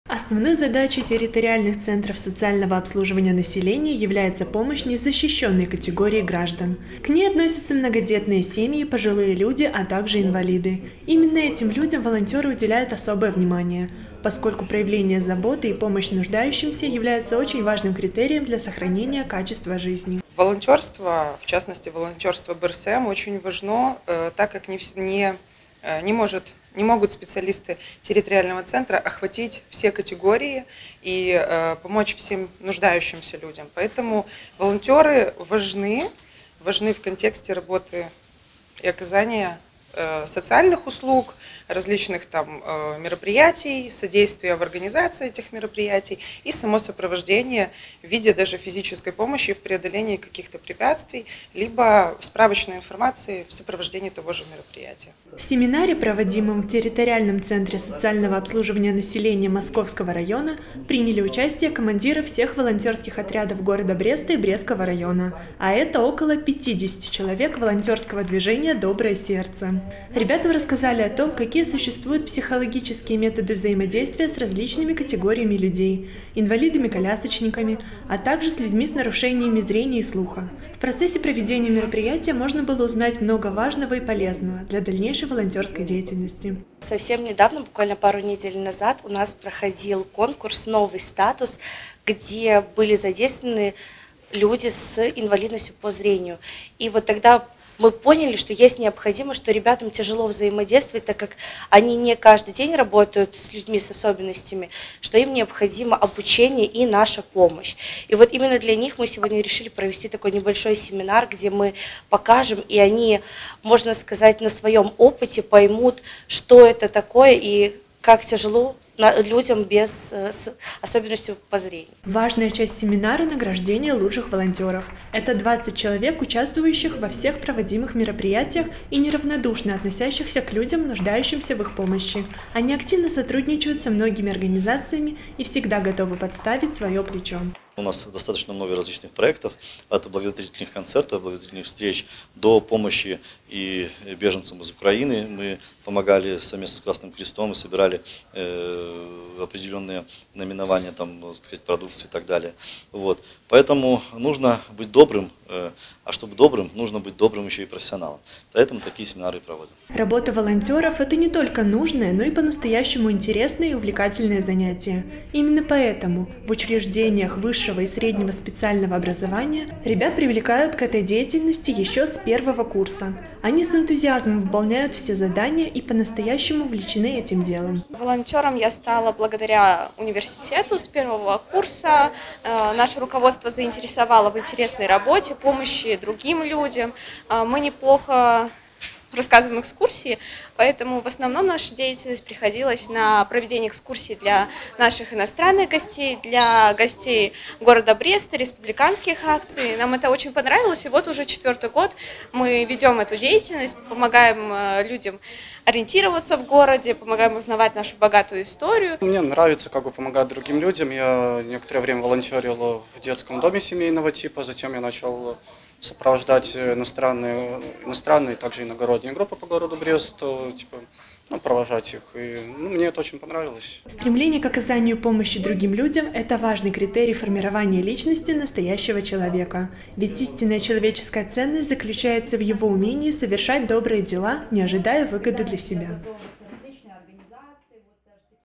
2022-09-08 г. Брест. Семинар для волонтеров в ТЦСОН Московского района. Новости на Буг-ТВ.
Журналист Н. Хомич. Оператор Э. Бакунович
«Чтобы делать добро, надо прежде всего им обладать» - именно с этой цитатой великого философа Аристотеля не могут не согласиться командиры волонтерского отряда, посетившие семинар, прошедший накануне в территориальном центре социального обслуживания населения Московского района города Бреста. Ведь для того, чтобы помогать людям необходимо чуткое сердце и непоколебимое желание, а ребята-волонтеры уже не раз доказывали, что обладают всеми необходимыми качествами. Именно для них проводилось мероприятие, которое было посвящено 20-летию со дня образования БРСМ, что является очень значимой датой для всех неравнодушных людей.